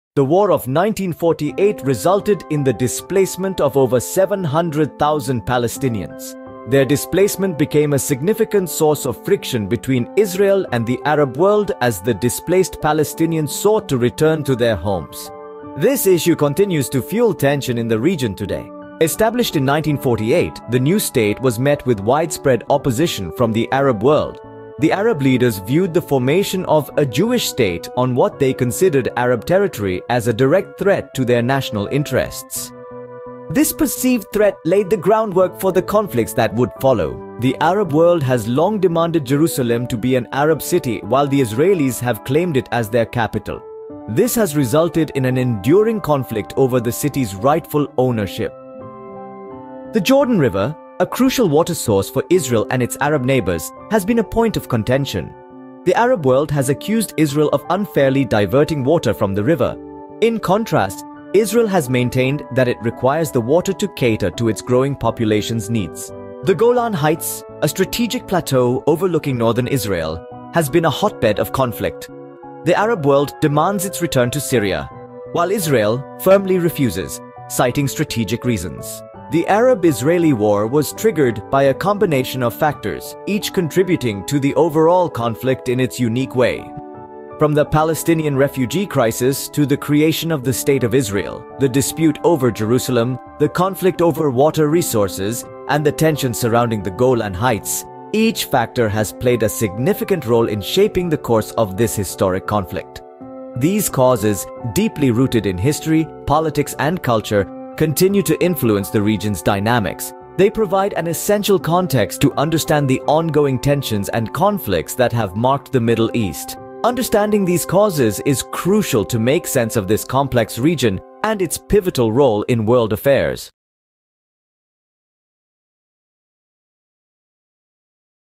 The 5 Main Causes of the Arab-Israeli War
The Arab-Israeli War was a series of wars fought between Israel and its Arab neighbors from 1948 to 1973. The wars were caused by a number of factors, including:

The creation of the State of Israel: The creation of Israel in 1948 was met with widespread Arab opposition. Arab leaders saw the establishment of a Jewish state in what they considered to be Arab territory as a threat to their own national interests.

The Palestinian refugee crisis: The 1948 war resulted in the displacement of over 700,000 Palestinians. These refugees became a major source of tension between Israel and the Arab world, as they demanded the right to return to their homes.

The Arab-Israeli conflict over Jerusalem: Jerusalem is a holy city to both Jews and Muslims. The Arab world has long demanded that Jerusalem be an Arab city, while Israel has claimed it as its capital.

The Arab-Israeli conflict over water: The Jordan River is a vital source of water for both Israel and its Arab neighbors. The Arab world has accused Israel of unfairly diverting water from the river, while Israel has argued that it needs the water to meet the needs of its growing population.

The Arab-Israeli conflict over the Golan Heights: The Golan Heights is a strategic plateau that overlooks northern Israel. Israel captured the Golan Heights from Syria in the 1967 war. The Arab world has demanded that Israel return the Golan Heights to Syria, while Israel has refused.

(2) İsrail-Filistin: Sosyal medyada etiketler nasıl savaşıyor? - BBC.